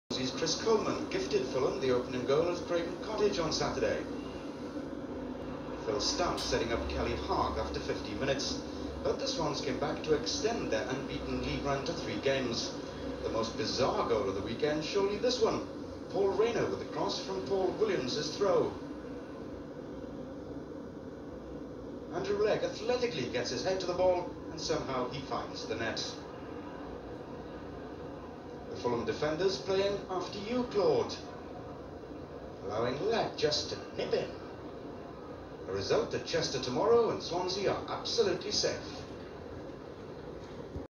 Fulham 1-1 Swansea City 20/4/91 at craven cottage. Andy Legg goal.
the swans draw 1-1 at craven cottage in their red spall Dp away kit thanks to an Andy Legg header.